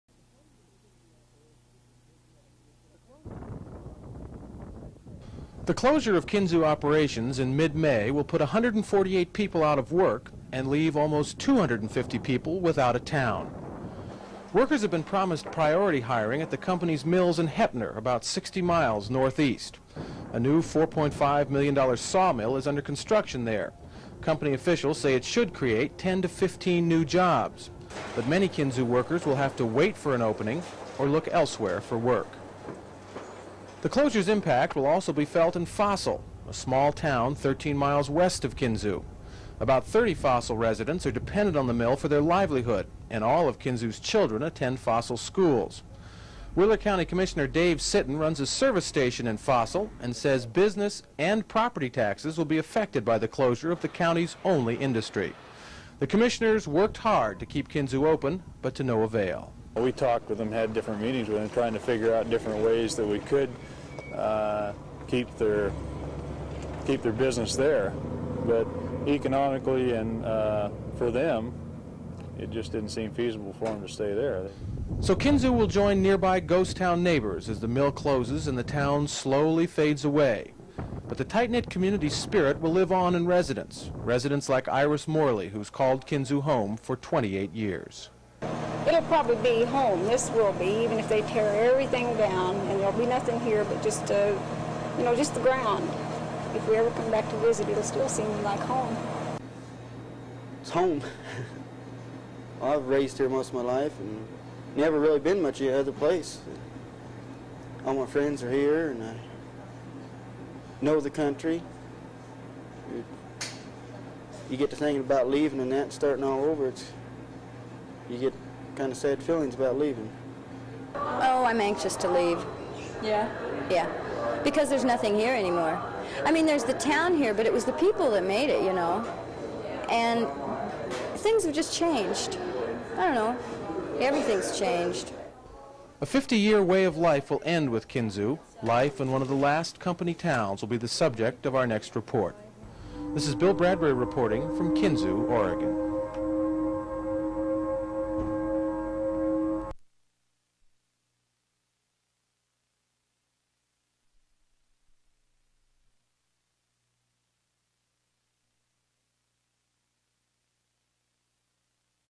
Kinzua Lumber 2 of 4
The end of company-owned mill towns in Oregon